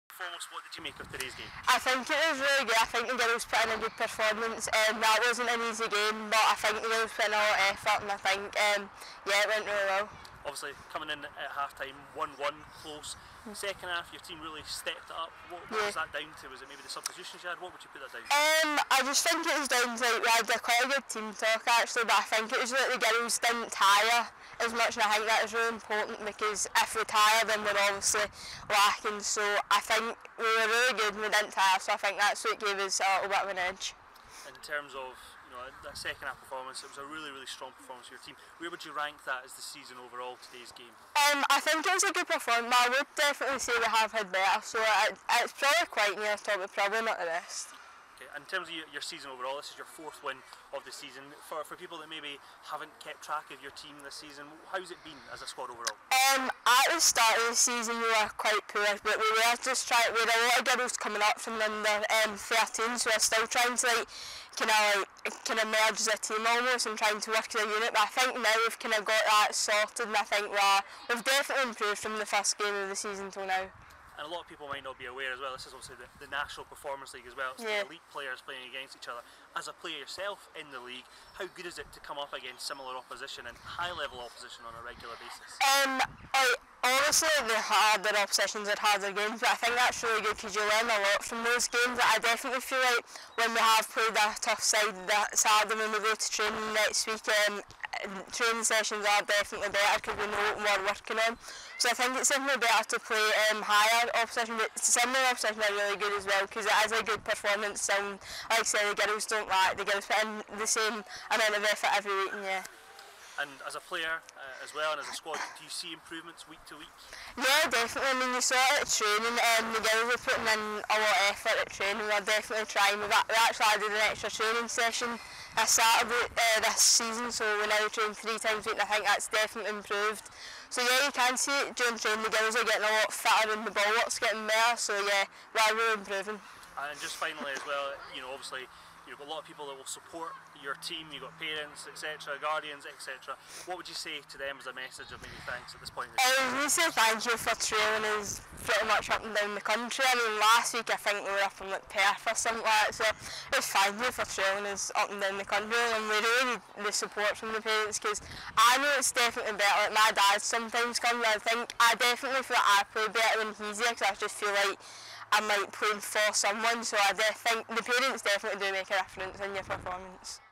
U15's National Performance League - Post Match Interview - FC Kilmarnock
We spoke to FC Kilmarnock U15's Forward, Tiegan Ferrie, after their 3-1 win over Bayside Girls on Saturday 15th September 2018.